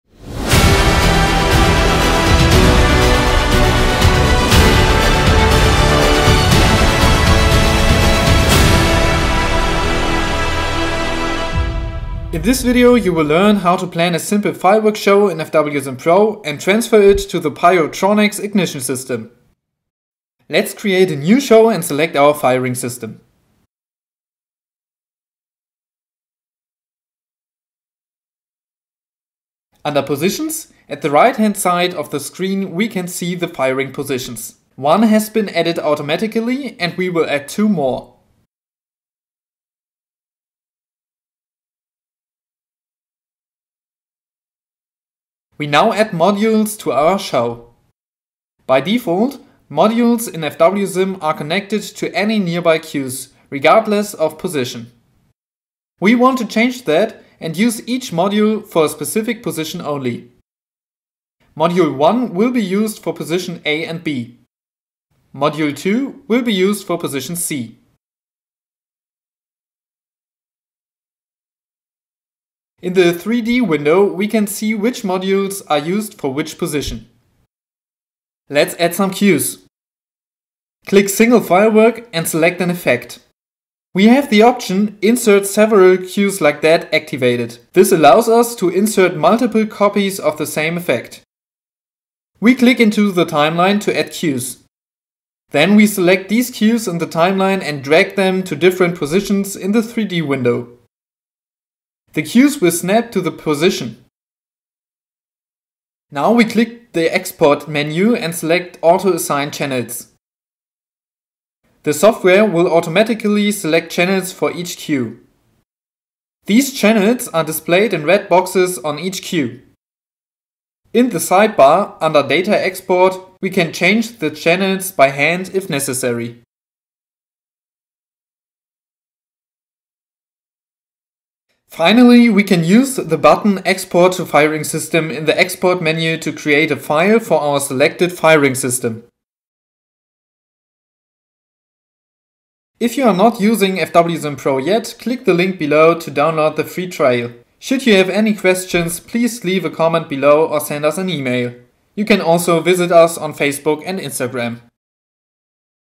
Pyrotronix Firing System: How to plan your show in FWsim Pro 🔥🎇
This video shows how to use FWsim Pro to plan a simple fireworks show and create firing data for the Pyrotronix firing system.

FWsim Pro allows you to easily visualize your fireworks shows. Pyrotechnicians use FWsim Pro to create preview videos for their clients, exchange ideas with colleagues and work on new effect designs. The software also allows you to create firing plans and export your shows to a variety of firing systems.

0:00 Intro
0:21 Adding Positions & Modules
1:19 Adding Cues
1:46 Assigning Channels
2:12 Export File
2:22 Outro